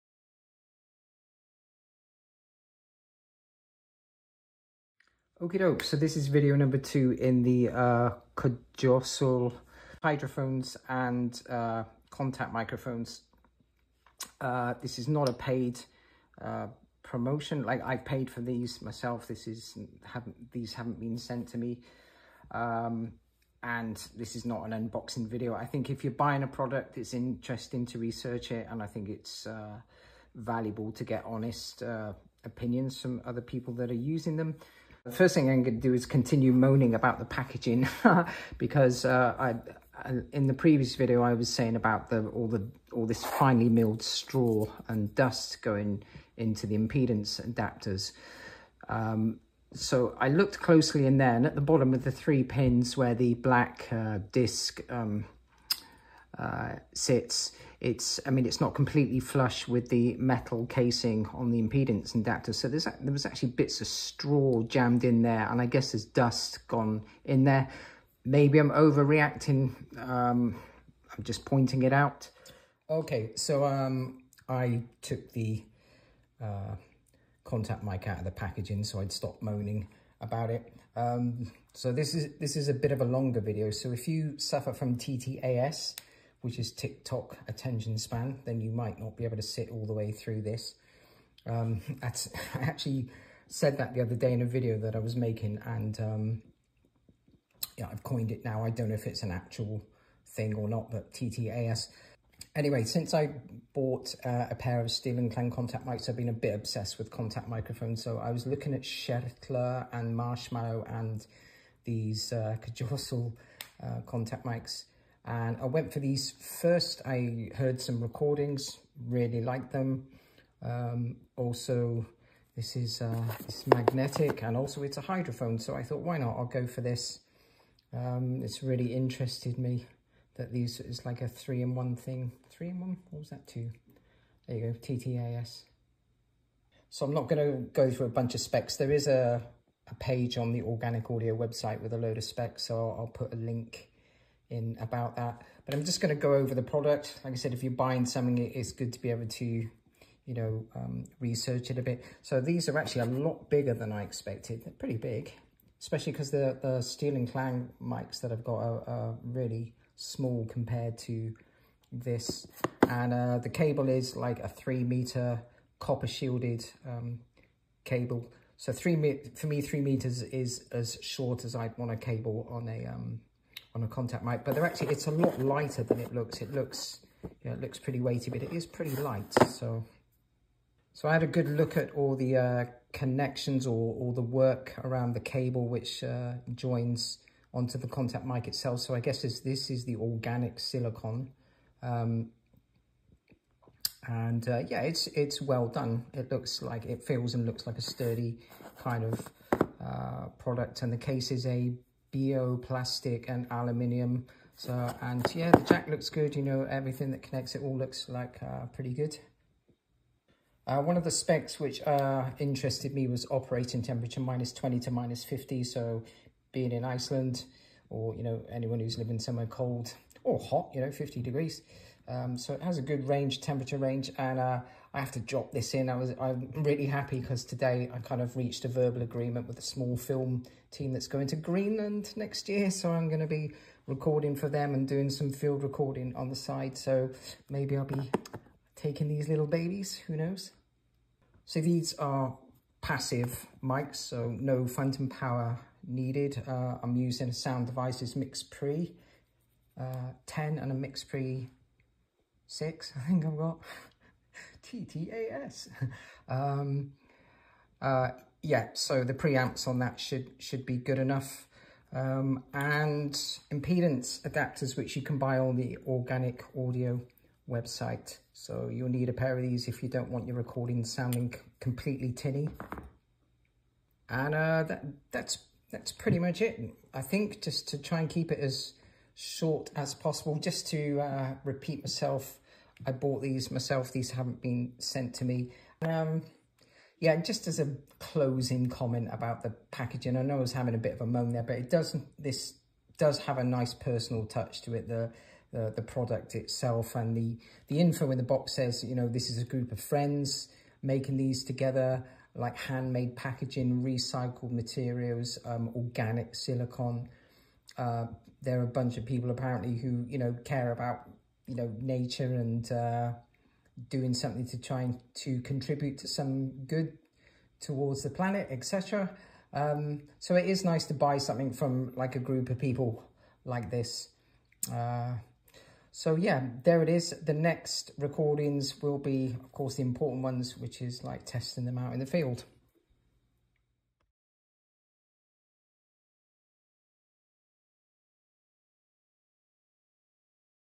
Organic Audio Cjossul V2 Magnetic Contact Microphone and Hydrophone - Part 02 - First Impressions
This is not an unboxing video just my first impressions of the Organic Audio Cjossul Contact / Hydrophones